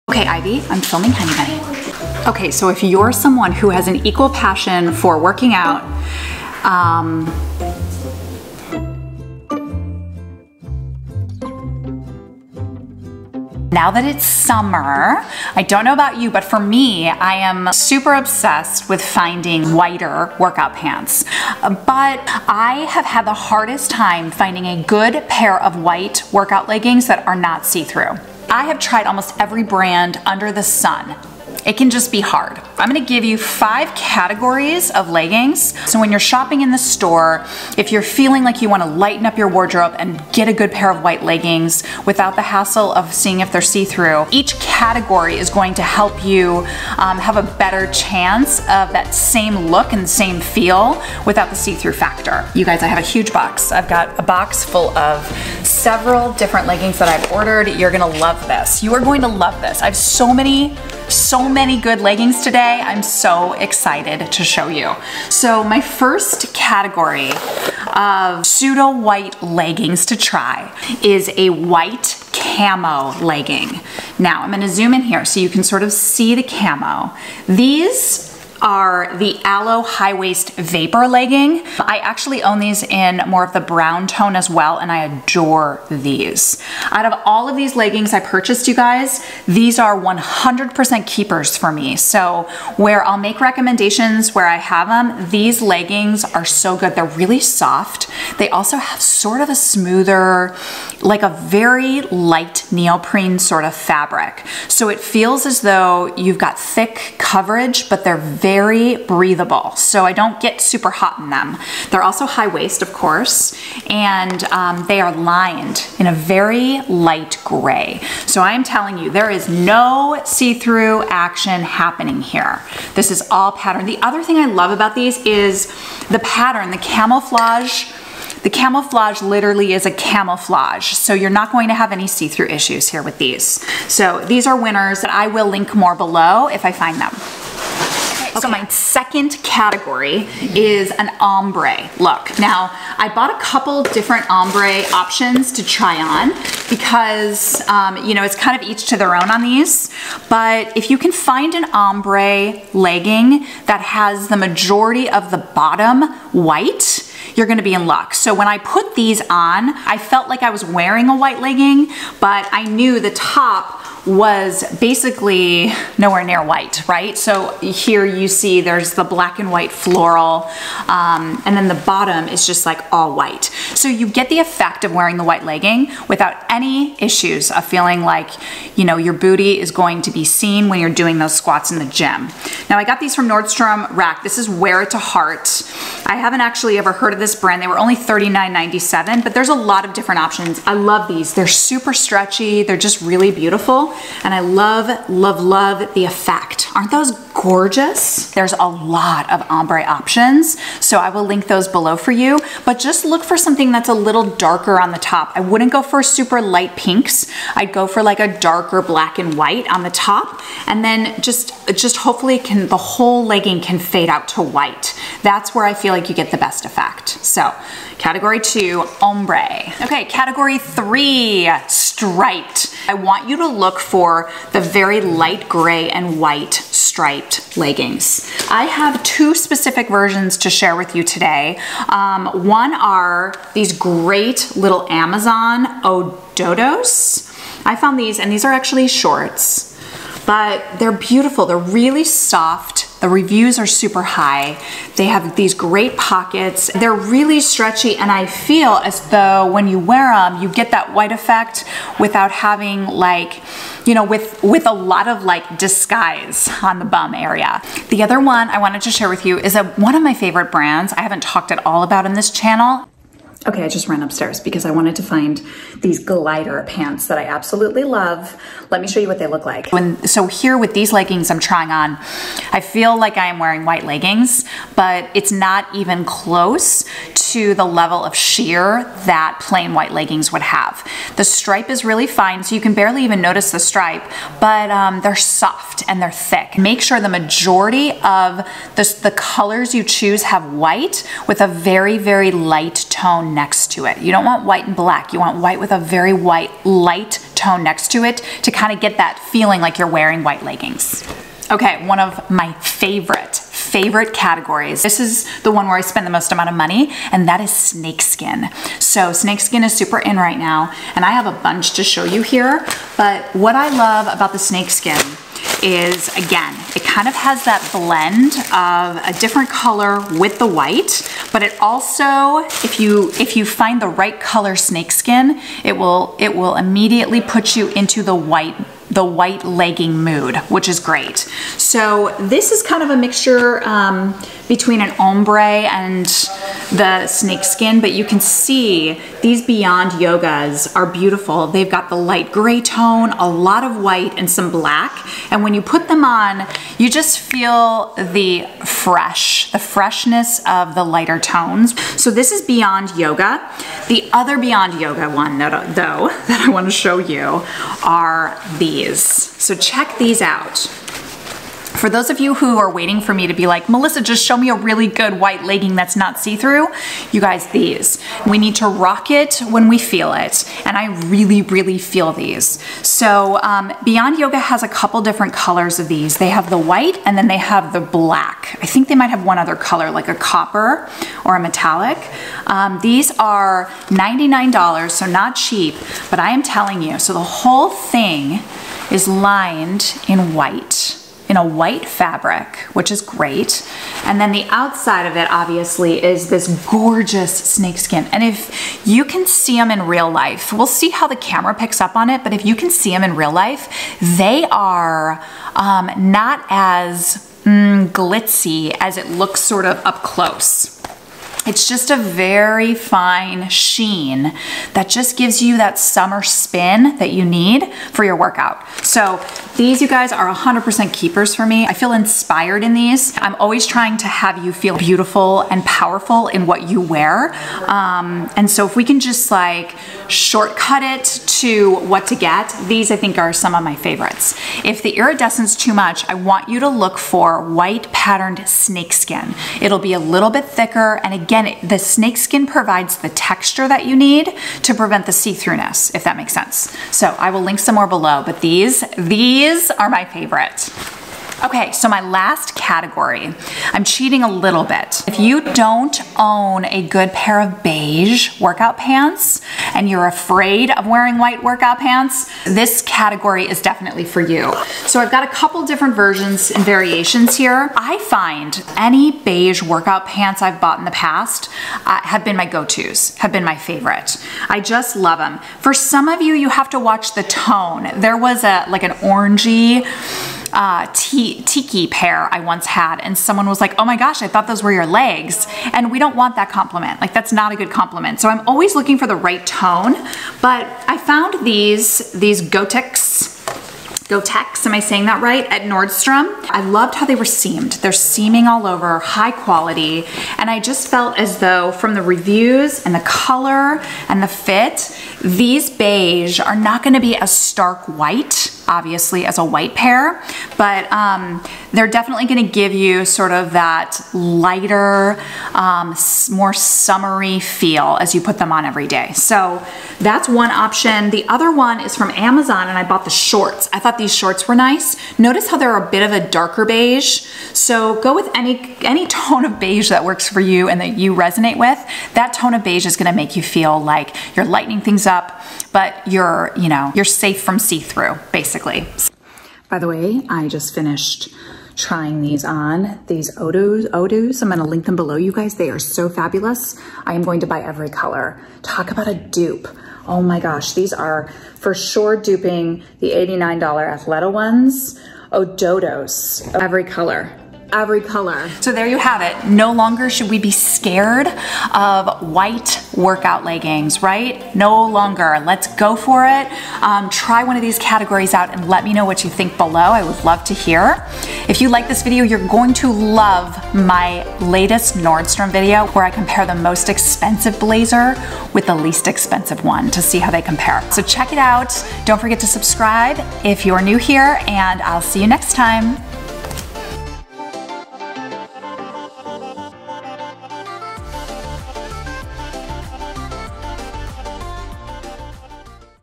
The Best ANTI SHEER White Yoga Pant Alternatives | AFFORDABLE + BEAUTIFUL!!
I love wearing white yoga pants - especially in summer! It's so hard though to find a good pair that doesn't show through. UNTIL NOW. I've searched high and low for the best white-yoga-pant alternatives, and here we are! I've got 5 categories for you here today - and just know that there's something here for everyone!

LINKS TO MY FAVORITE PANTS IN EACH CATEGORY!
Category 1 (CAMO):
1: Alo (my fav! as seen in the video)
2: Lululemon
3: Lululemon DUPE
4. Fav brand- soft and thick!

Category 2 (OMBRE):
1: Ombre Amazon Favorites

Category 3 (STRIPES):
1. Shorts (as seen in video)
2. Glyder
3. Glyder as seen in video
4. Lululemon
5. Lululemon dupe

Category 4 (SNAKESKIN):
1. FAVS! (as seen in video)
2. Pure White Snakeskin (as seen in video)
3. Metallic White
4. DUPE!

Category 5 (BEIGE):
1. Best DUPE shorts!! (in color dark beige and space dye brown)
2. Favs